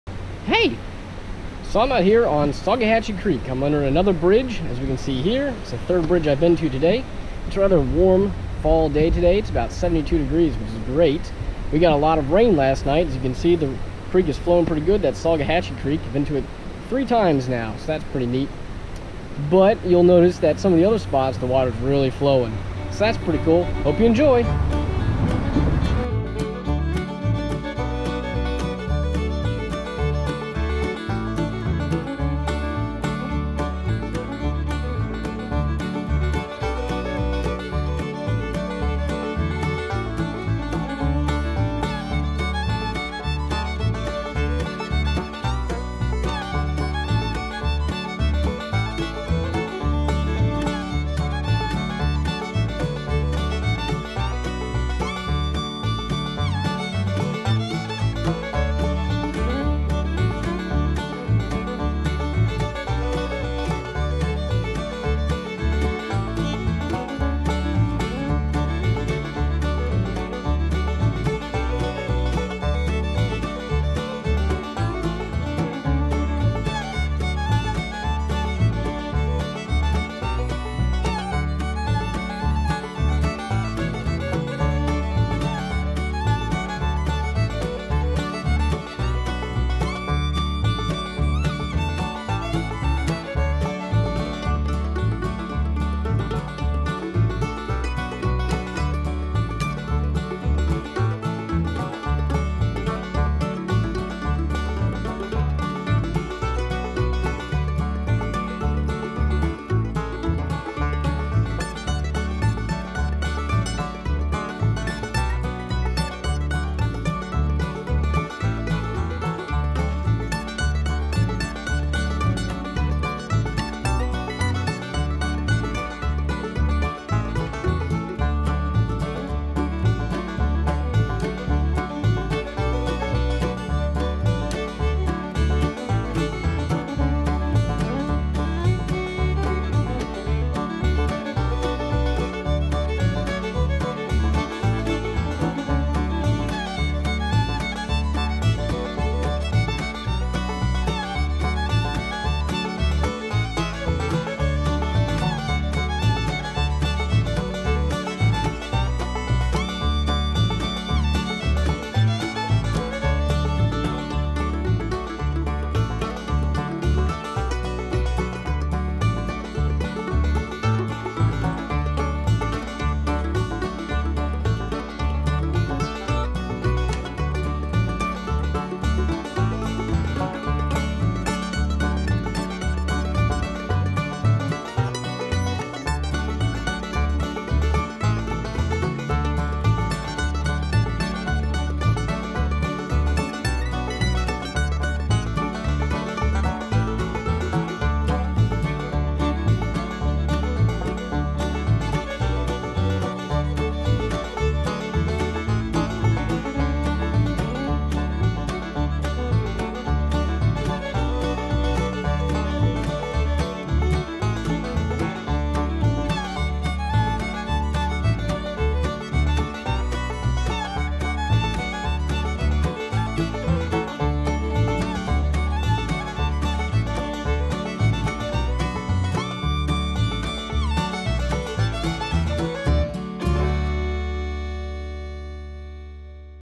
Three Times on Sougahatchee Creek | DJI Mavic Air
I found three different places along Sougahatchee Creek to fly today, so I did. All had bridges, and some good rocky exposures, however the rain last night might have obscured them.